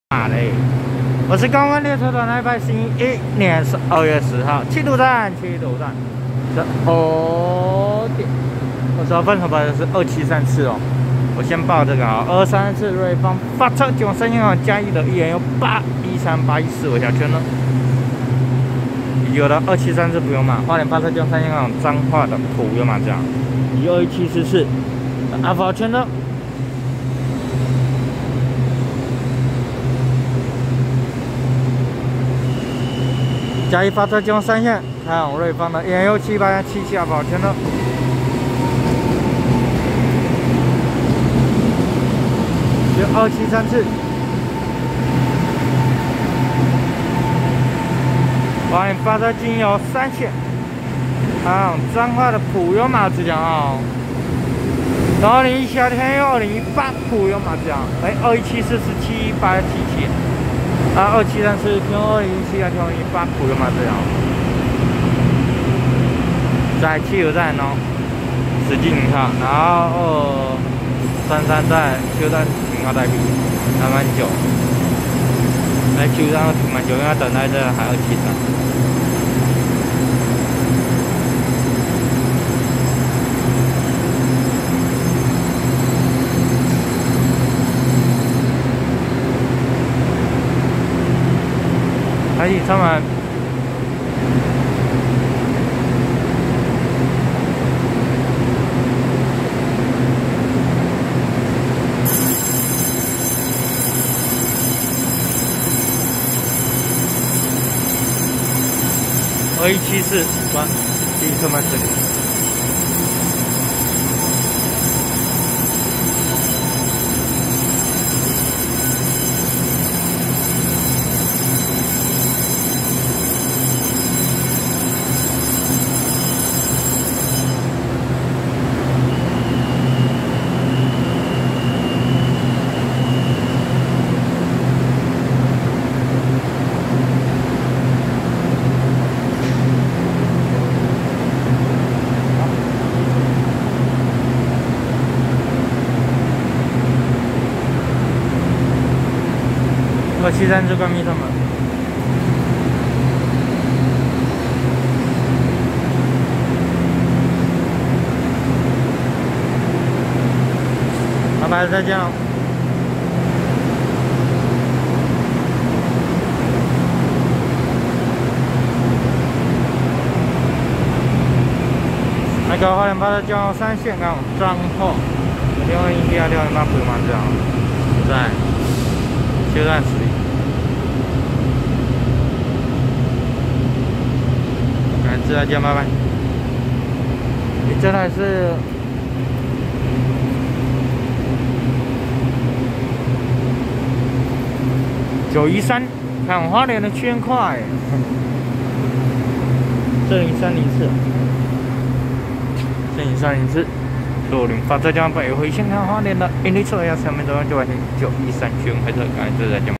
2233次瑞芳發車經由宜蘭線開往嘉義的EMU813+EMU814微笑號區間車，2174次嘉義發車經由山線開往瑞芳的EMU718+EMU717阿福號區間車
111/12/10七堵站15:22分拍攝2233次瑞芳發車經由宜蘭線開往嘉義的EMU813+EMU814微笑號區間車，2174次嘉義發車經由山線開往瑞芳的EMU718+EMU717阿福號區間車